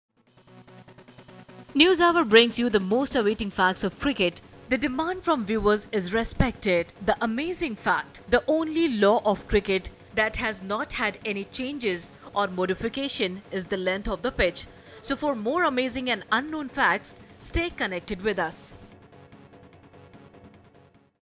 Laws of Cricket: Length of Cricket Pitch
SPORTS HOUR brings viewers Unknown Facts. Length of the pitch has not had any changes.